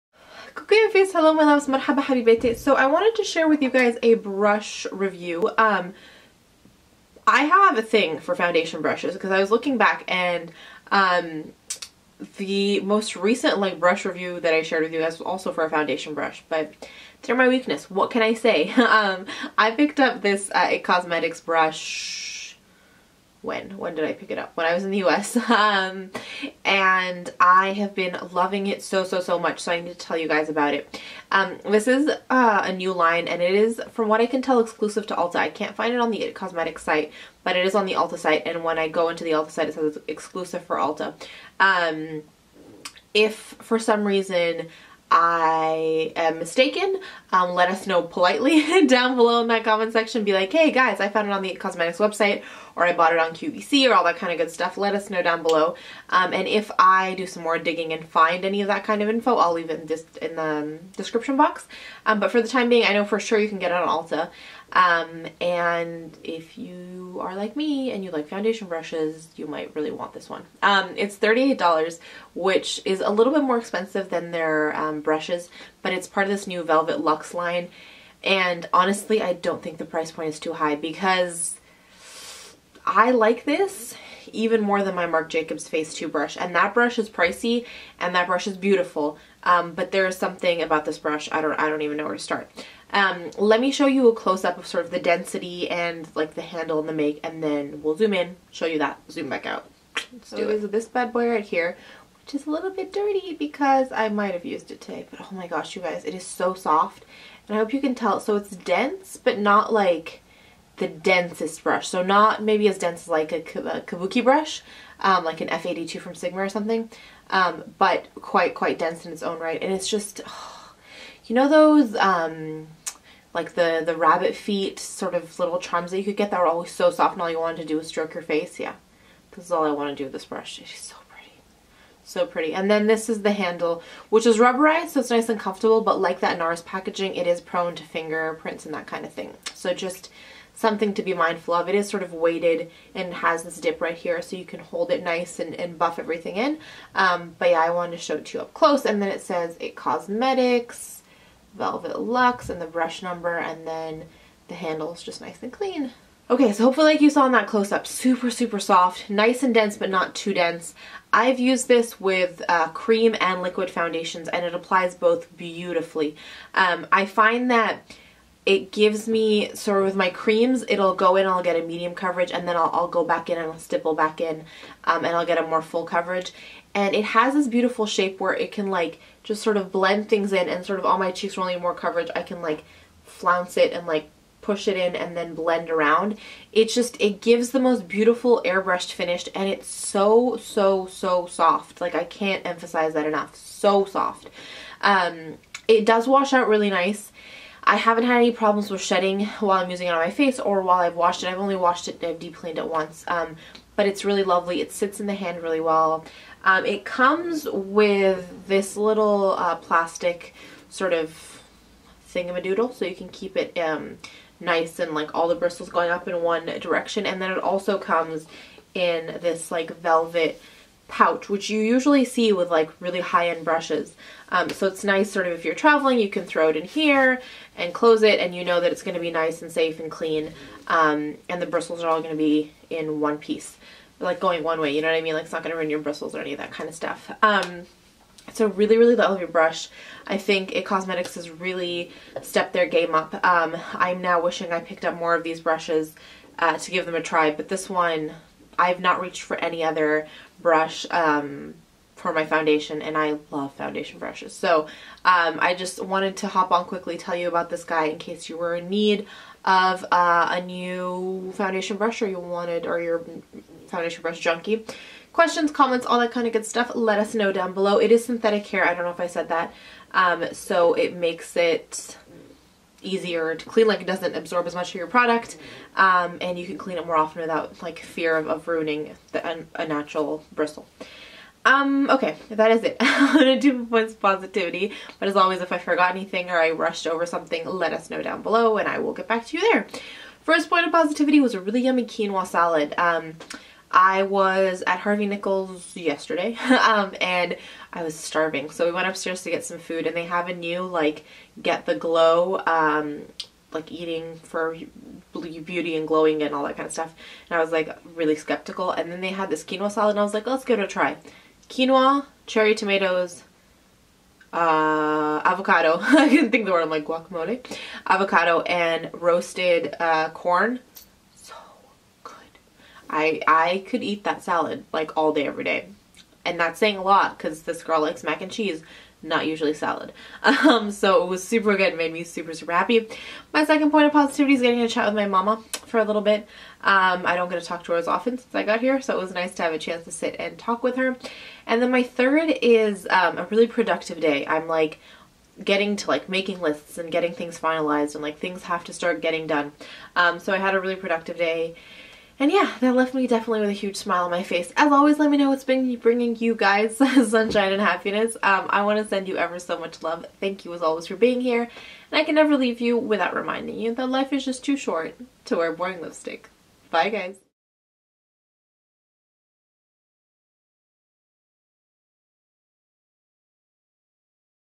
Product Rave: IT Cosmetics Velvet Luxe LBD #302 Brush
What I’m Wearing:

Eyes - elf Beauty Elsa Beauty Book, MAC Fluidline in Phlox Garden, Tarte Light, Camera, Flashes Mascara

Face - Dior Pore Minimizing Primer, NARS Sheer Glow in Fiji, Chantecaille Capri Bronzer, Makeup Forever Cream Blush in 410, The Balm Mary Lou-Manizer

Lips - NARS Audacious Lipstick in Anna